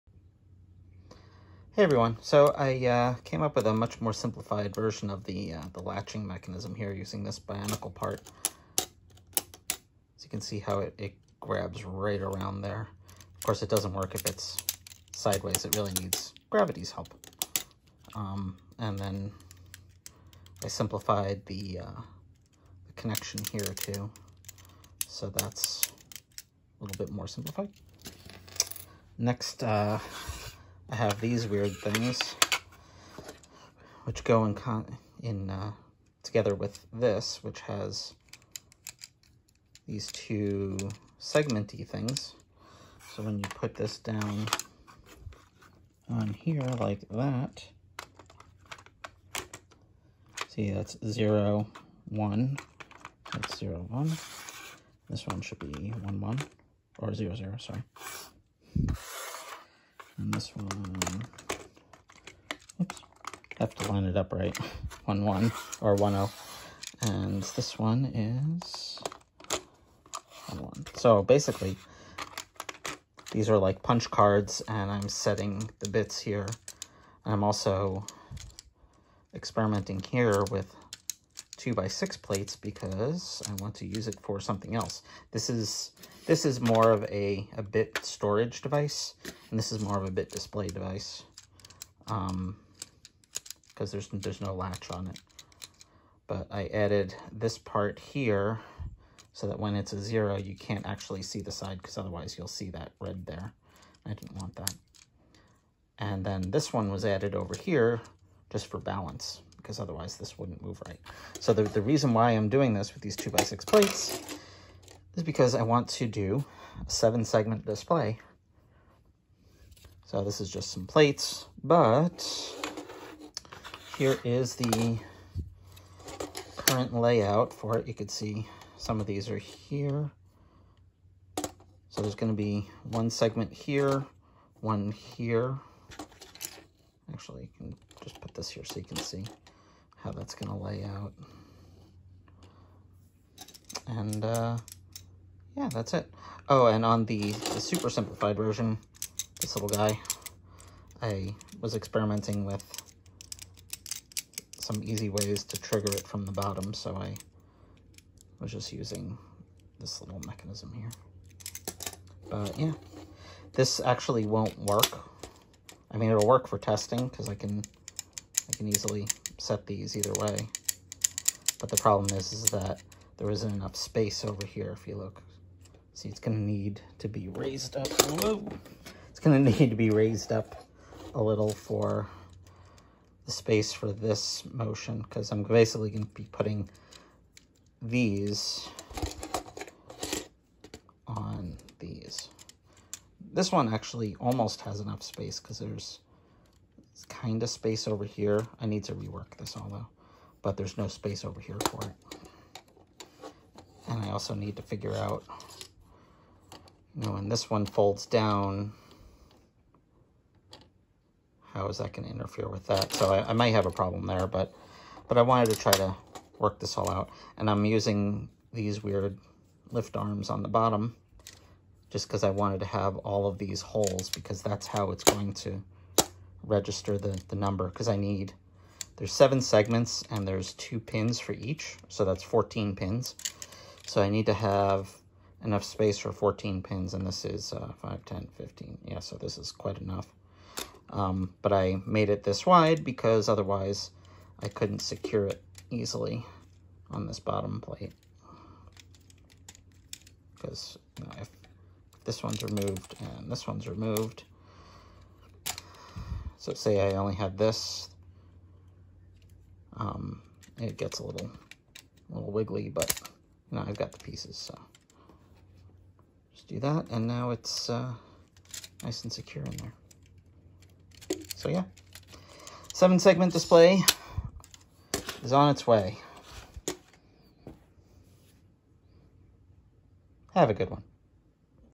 Technic Lego 7-segment display
Demoing the better version of the bit latch mechanism and the beginnings of a seven segment (one digit) display.